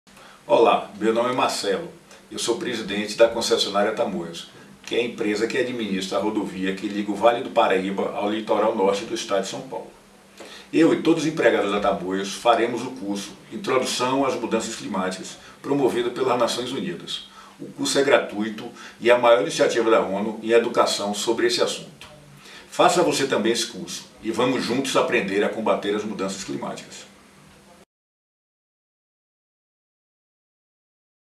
We will take the UN CC:Learn e-Course in Portuguese
Marcelo, CEO of Tamoios in Sao Paulo, Brazil, announced that he and all of his groups'employees will take the UN CC:Learn e-course "Introduction to Climate Change."

The course is available for free, and a participant receives a certificate  after completing the course.